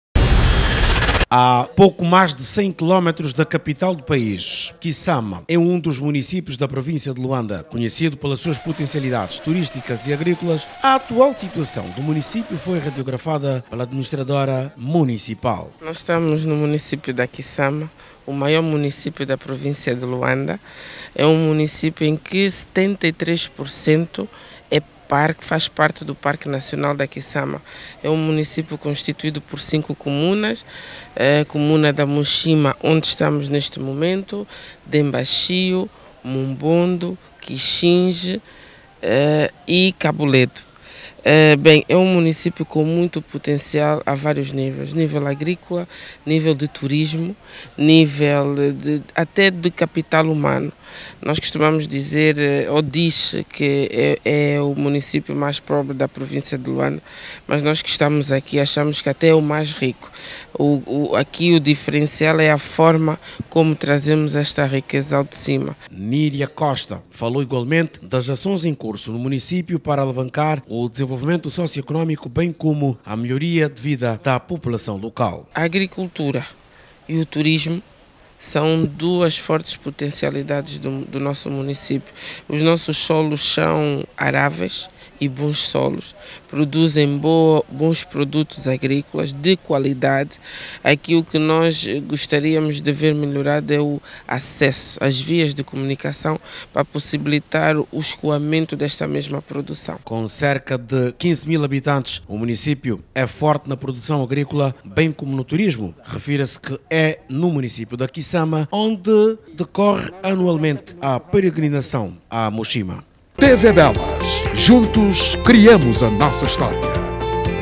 ADMINISTRADORA FALA DAS POTENCIALIDADES DO MUNICÍPIO DA QUIÇAMA
Há pouco mais de 100 quilômetros de distância da capital do país, Quiçama é um dos municípios da província de Luanda.
Conhecido pelas suas potencialidades, turísticas e agrícolas, a actual situação do município, foi radiografada pela administradora municipal.
Níria Costa, falou igualmente das acções em curso no município, para alavancar o desenvolvimento sócio-econômico bem como a melhoria de vida da população local.
Com cerca de 15 mil habitantes, o município é forte na produção agrícola, bem como no turismo. Refira-se que é no município da Quiçama, onde decorre a peregrinação à Muxima.Saiba mais no